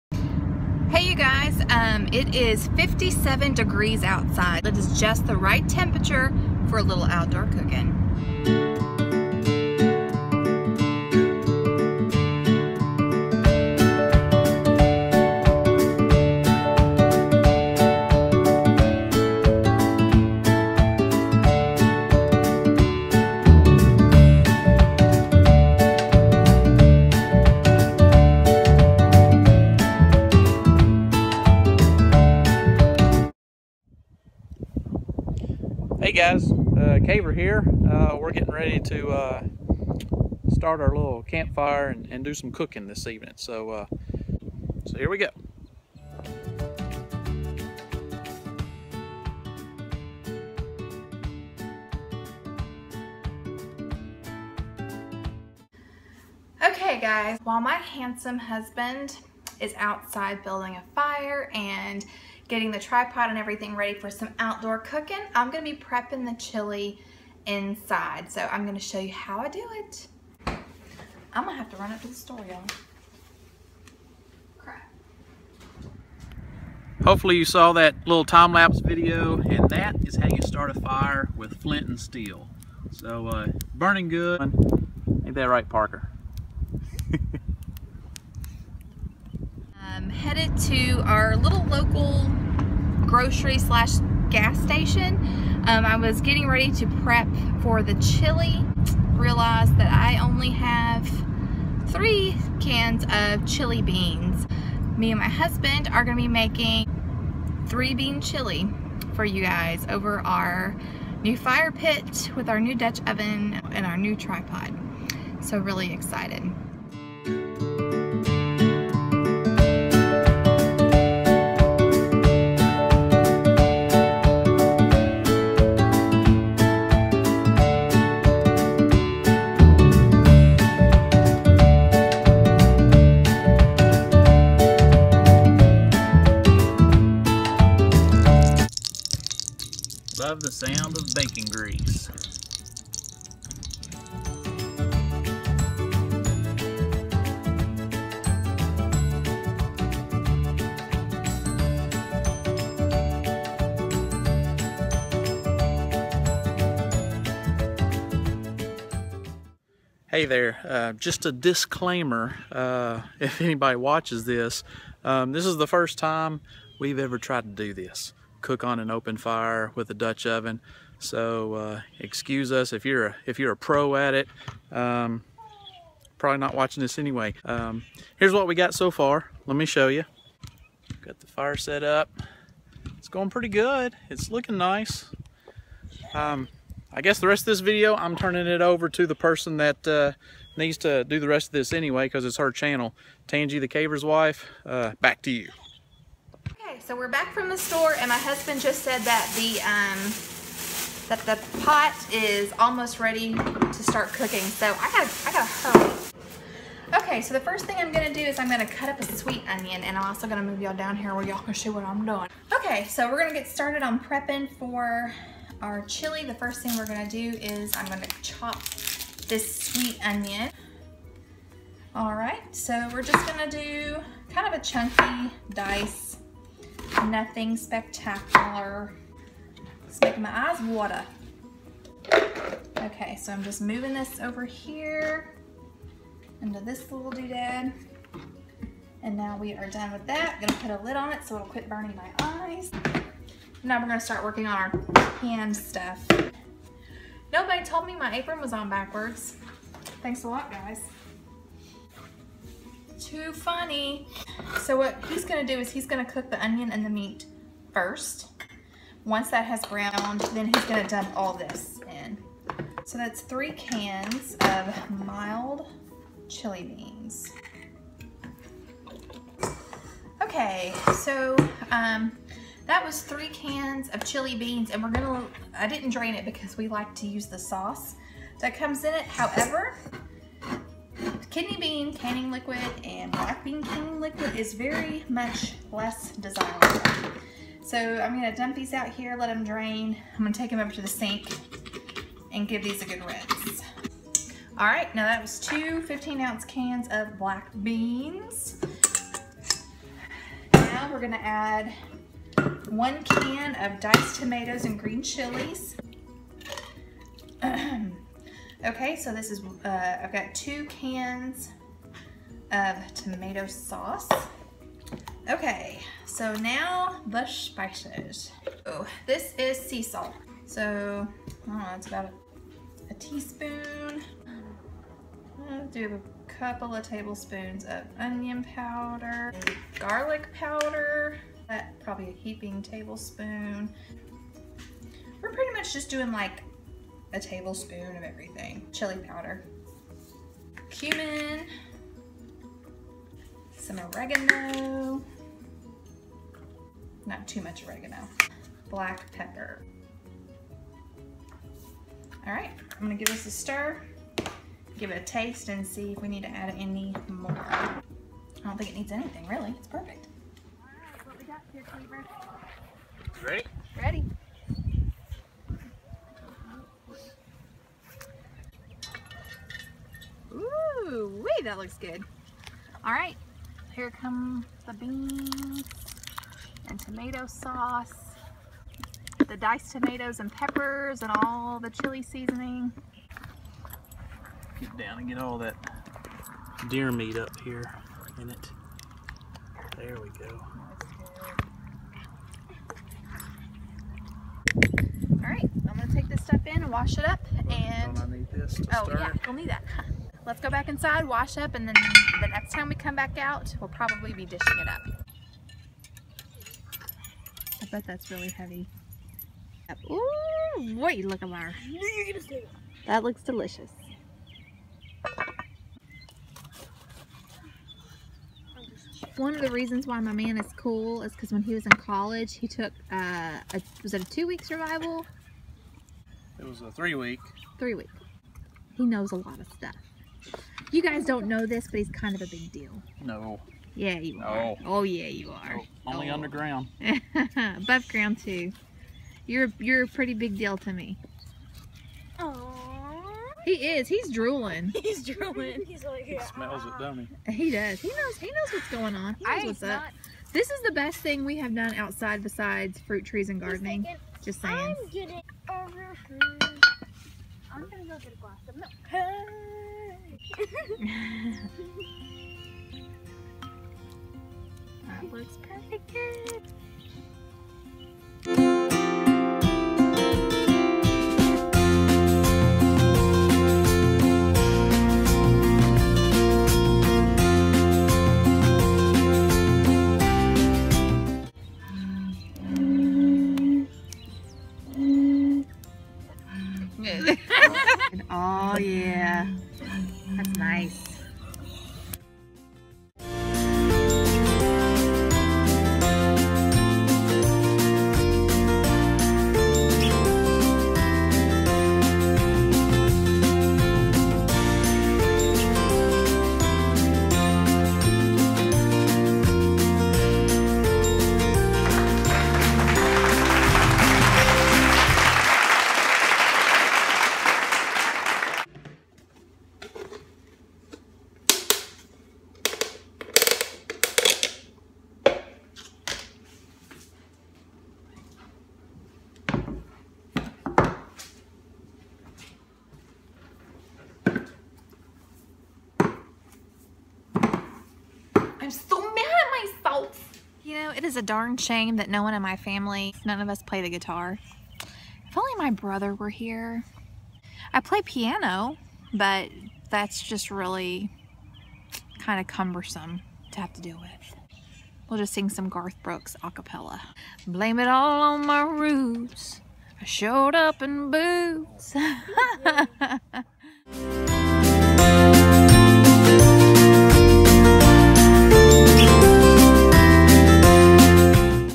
Dutch Oven Three Bean Chili
We are breaking in the new Dutch oven, fire pit, and tripod. Come along! You can grab the recipe on my blog,